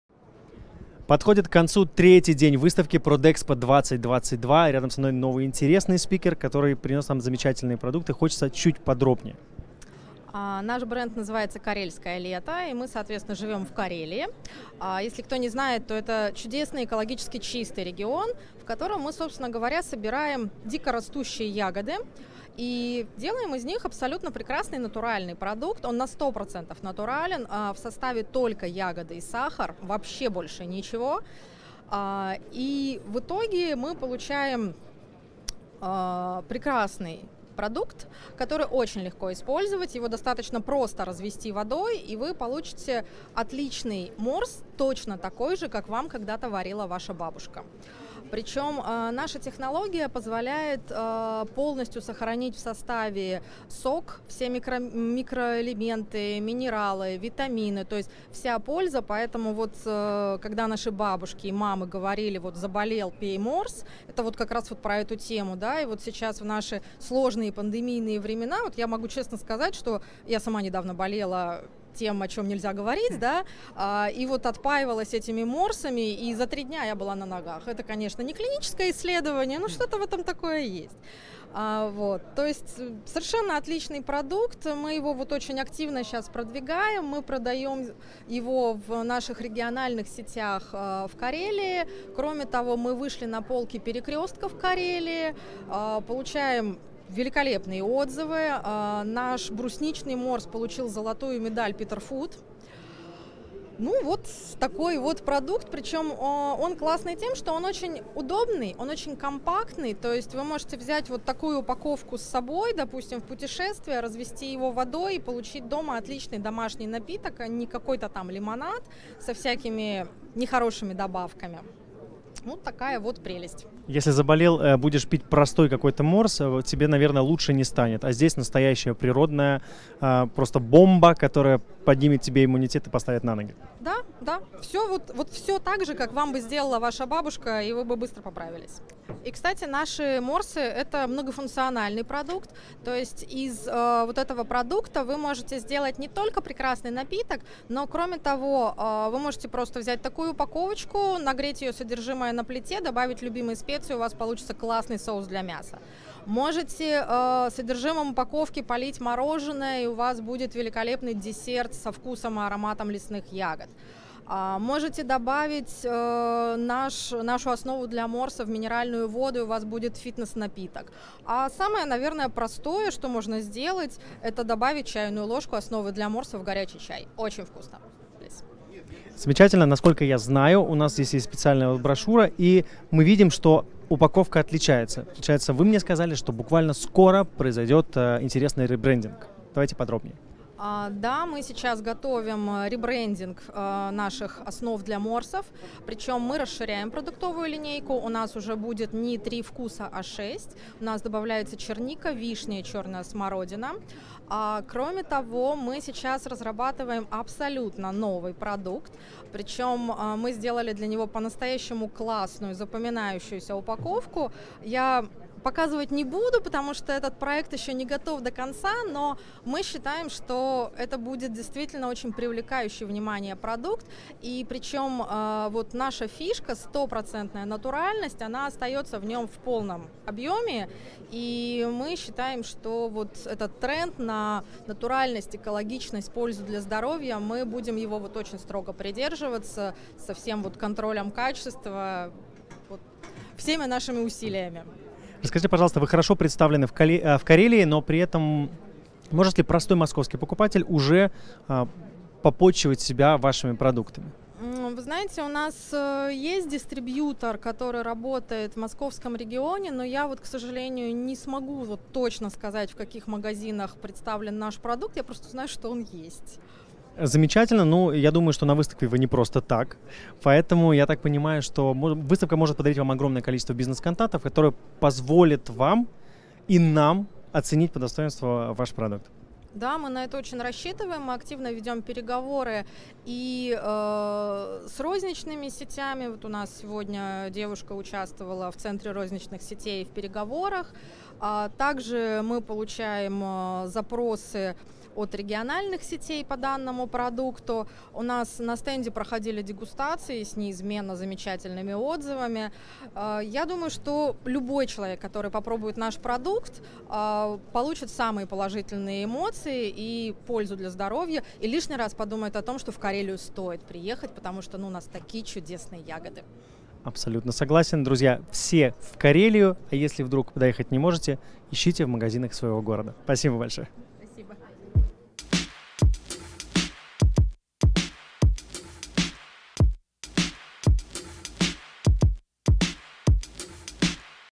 Лада Макарова - Карельское лето на
Лада Макарова - Карельское лето, о концентратах для приготовления морсов и о вариантах его использования, а также ребрендинге и новых вкусах продукции